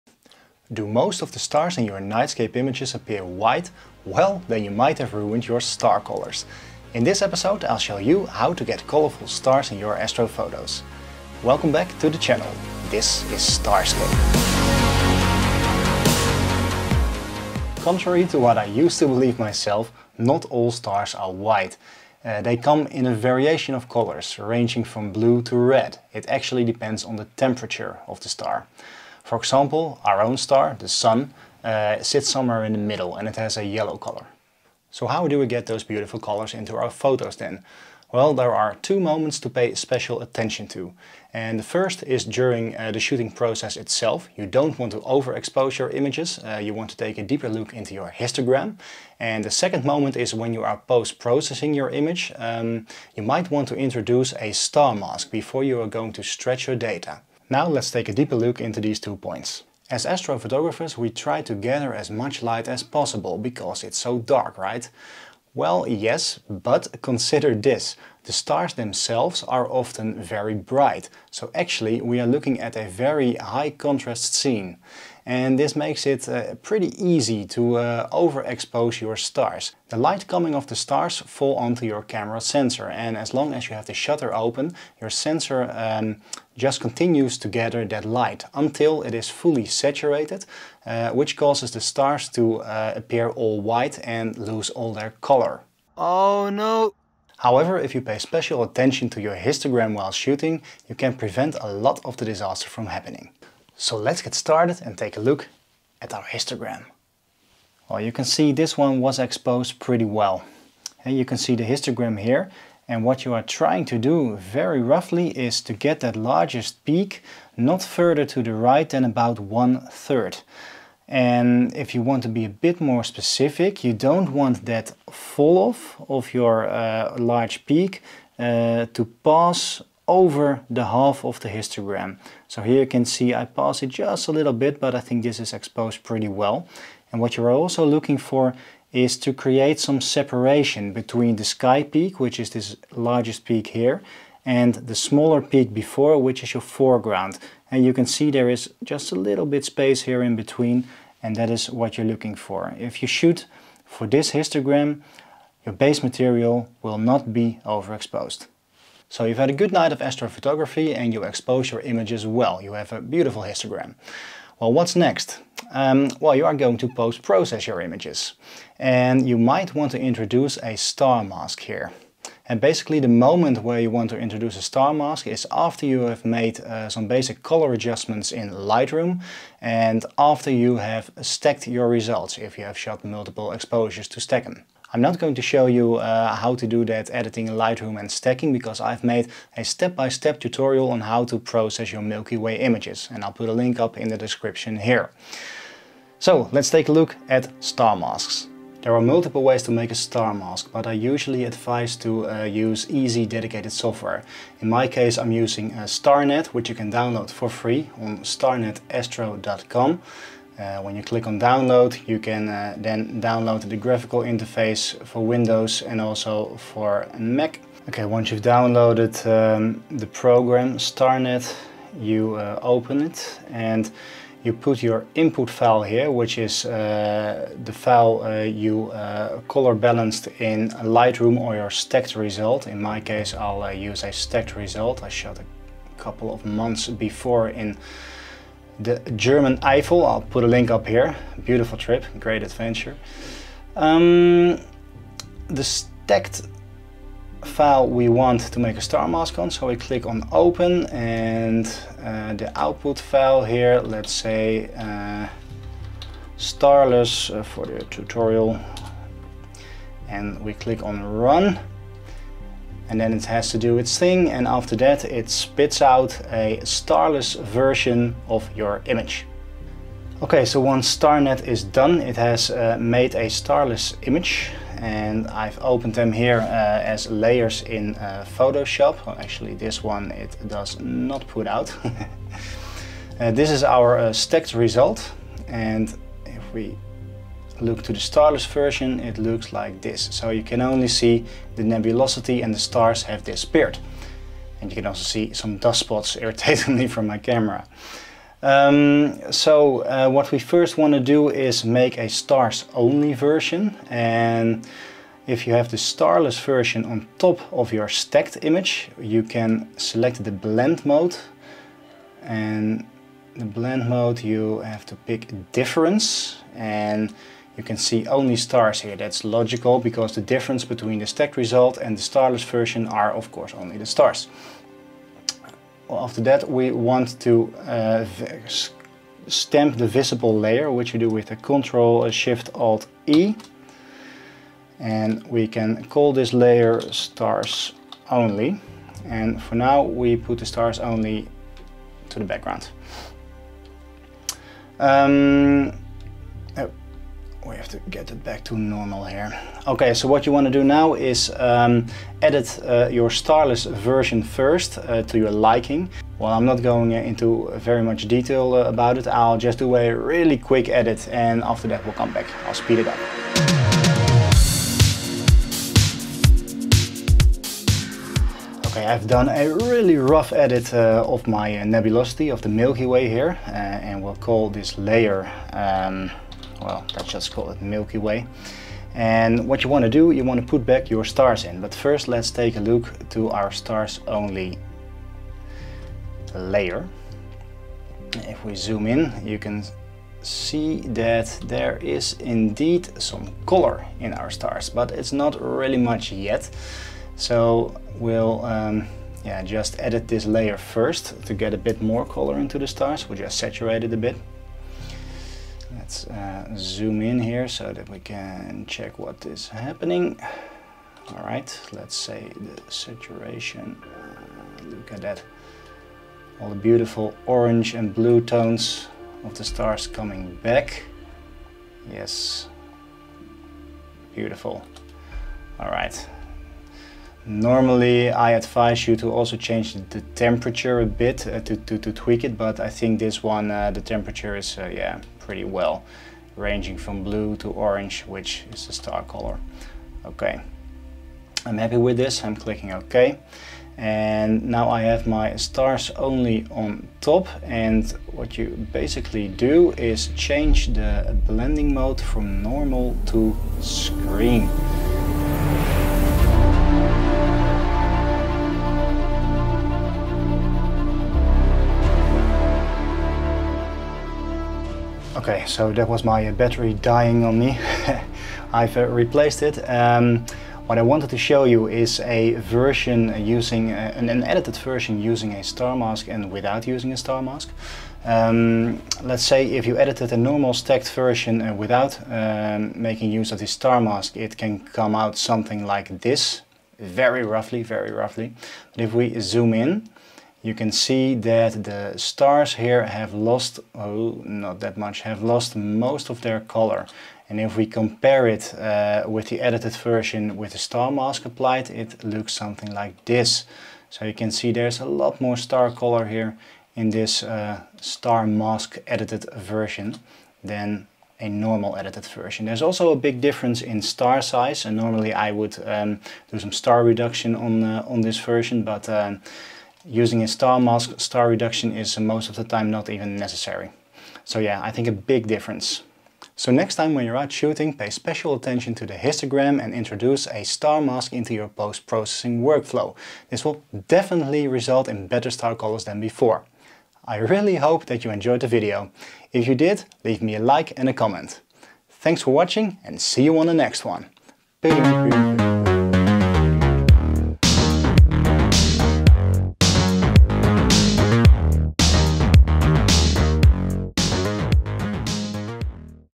Better STAR COLORS using Perfect Exposure (Histogram) and Star Masking (StarNet) in Astrophotography
Are most of the stars in your nightscape images white? Then you might have ruined your star colors! I show you how to preserve those beautiful star colors by using a correct exposure, looking closely to the histogram, and by using a star mask (StarNet) in your post processing workflow in Photoshop. A star mask makes it possible to edit the stars and the nebula / milky way (starless photo) separately.

00:00 Intro

00:17 Star Colors Introduction

00:59 Overexposing / blowing out the stars
01:37 Correct Exposure for Landscape Astrophotography (ideal histogram)

02:38 Star Mask Introduction
03:19 StarNet
04:30 Editing in Photoshop using a Star Mask
07:45 Comparison with and without Star Mask

08:46 Recap